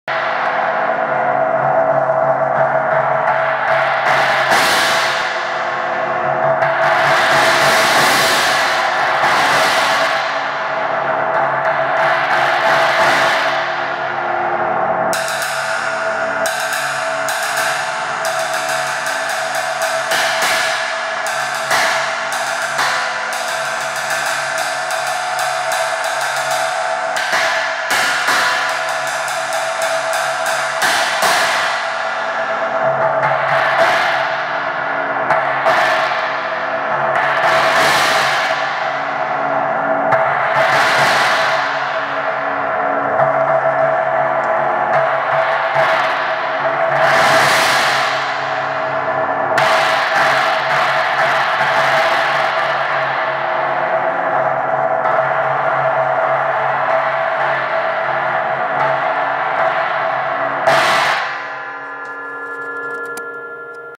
Wuhan China 28
This is a sound Test of the wuhan china. it is huge and heavy and has a slow reaction but it has lots of faccetes. its not monotous so i can say there are neaqrly unlimited sound options